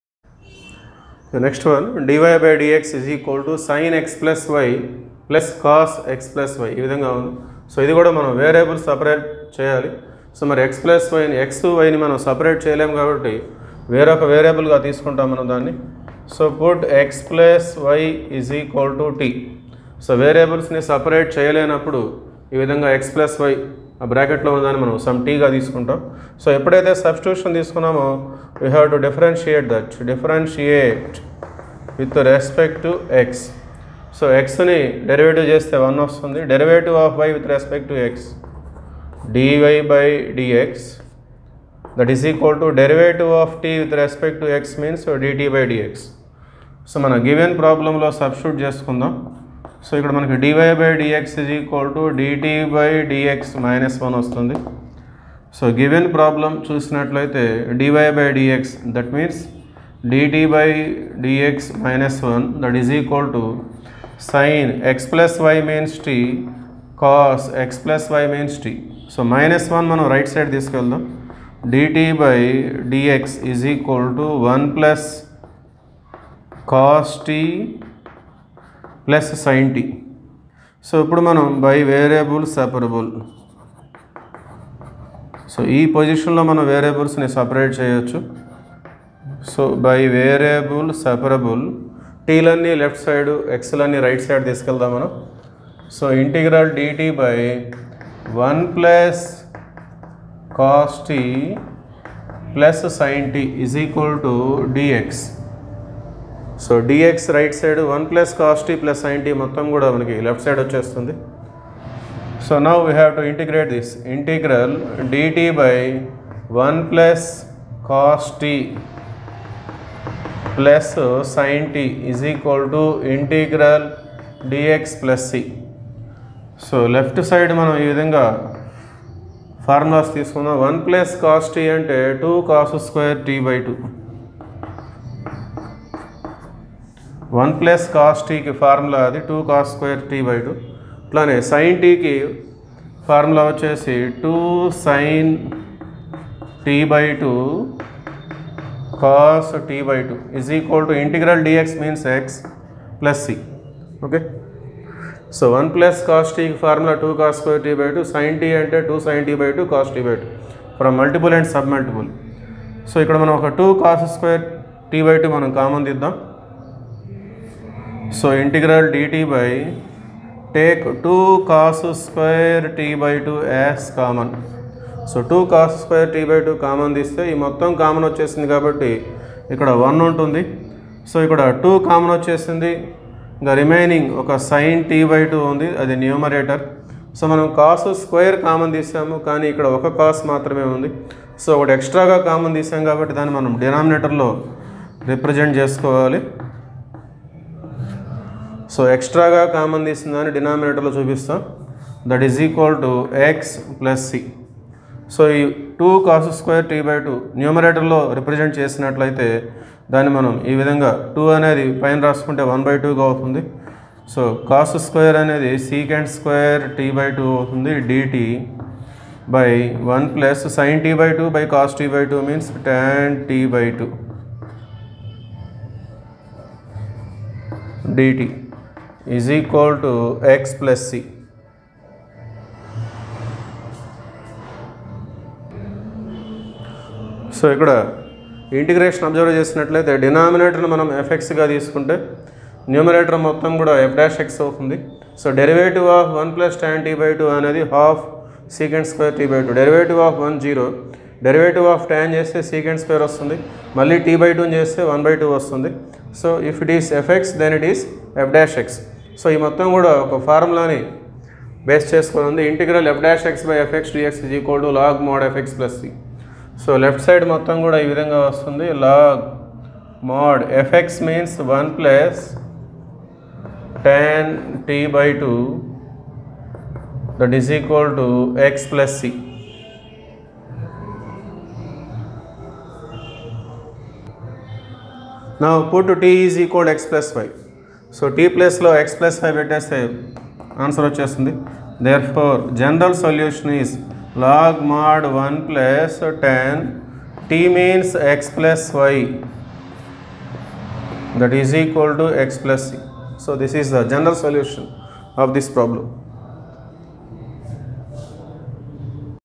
dy/dx = Sin (x + y) + Cos(x + y); | Variable Separable
Differential Equations | Variable Separable | 4 marks problem @EAG